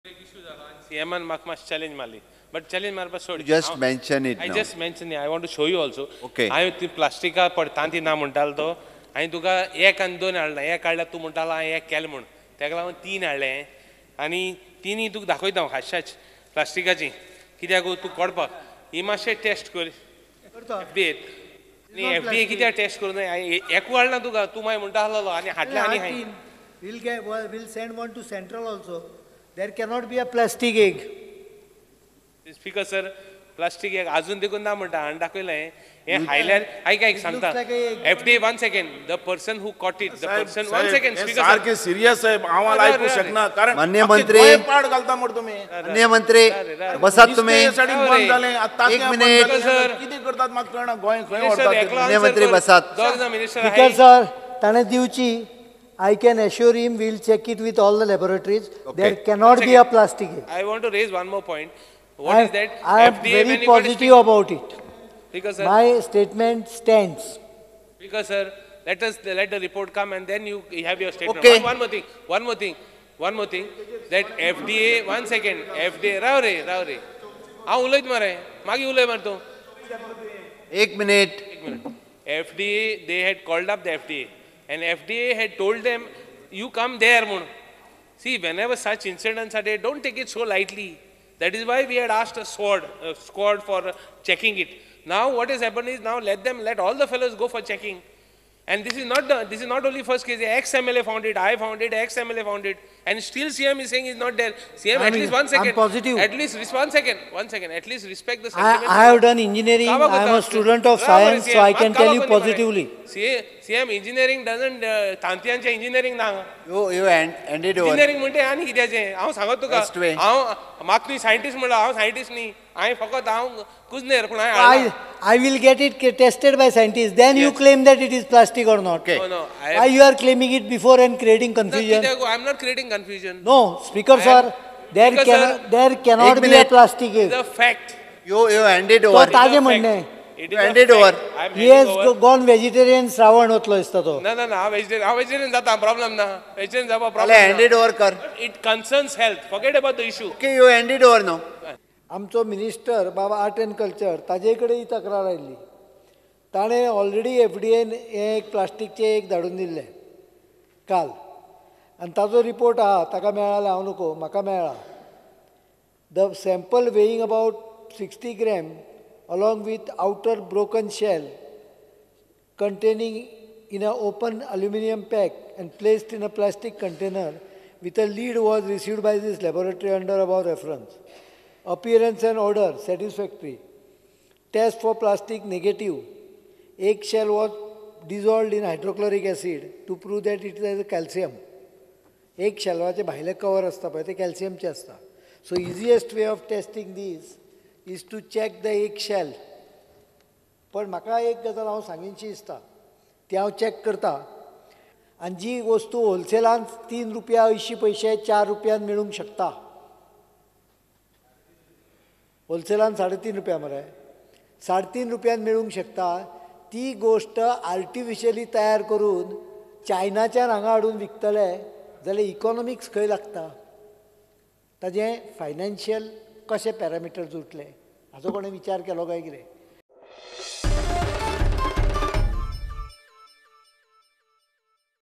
PLASTIC EGGS NOT ECONOMICALLY VIABLE & HUMANS ARE NOT AHEAD OF GOD: CM _Prudent Media Goa
Accepting challenge of Chief Minister Manohar Parrikar, Congress MLA Alex Reginald brought plastic eggs in Assembly. However, CM Parrikar stood by his word claiming the existence of plastic eggs in Goa to be false. He further assured the house to send it for testing to FDA and other labs for affirmation.